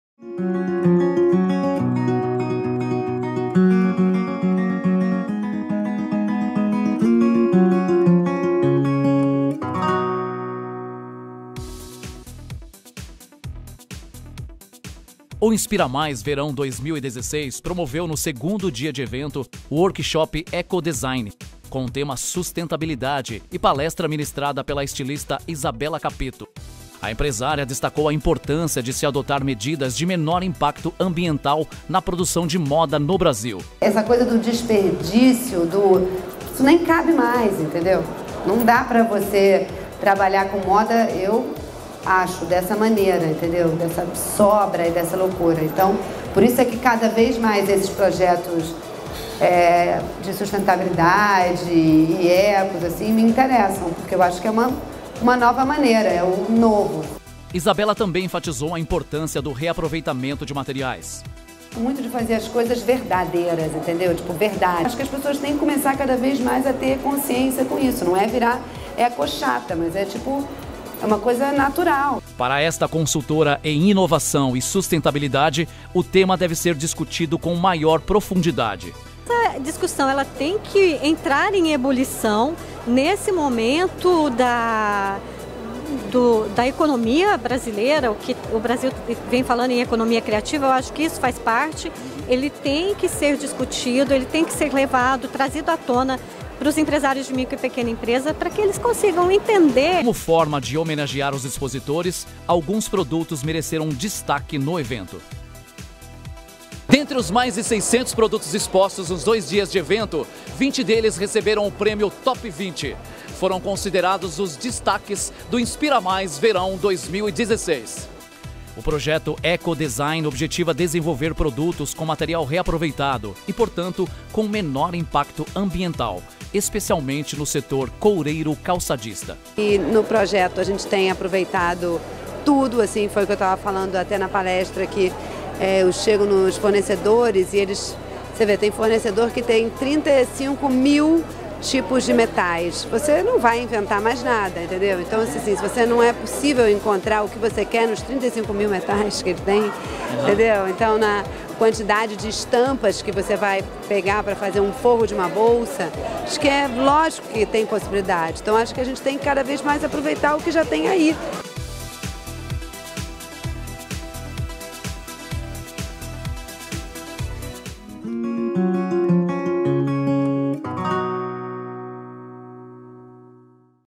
Desperdício não cabe mais, diz Isabela Capeto na InspiraMais
Estilista partipou do Workshop Ecodesign, que abordou materiais alternativos e práticas sustentáveis no mercado de design.